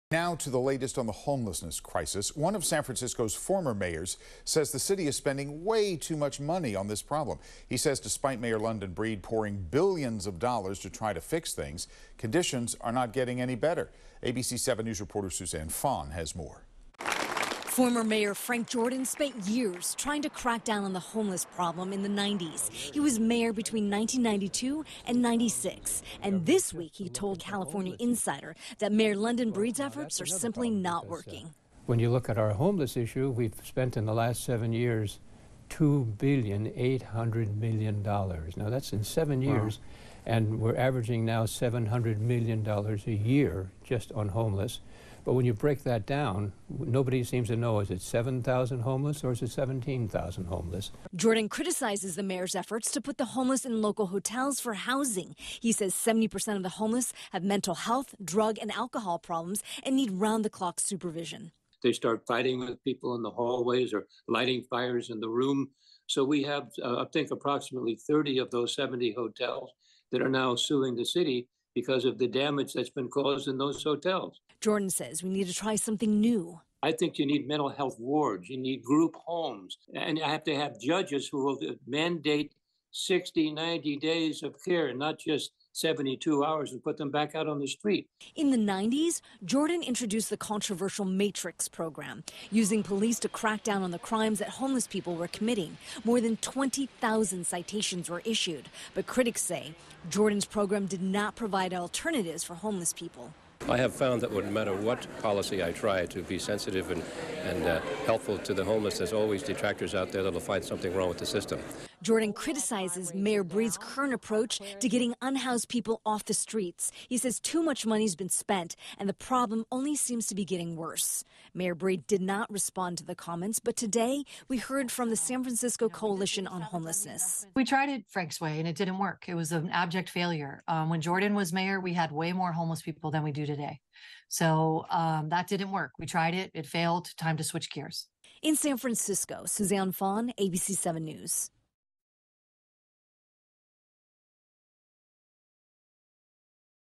Former San Francisco mayor criticizes London Breed's approach to solving homeless crisis
Former San Francisco Mayor Frank Jordan says despite Mayor London Breed pouring billions of dollars to try and fix the homeless crisis, conditions are not getting any better. STORY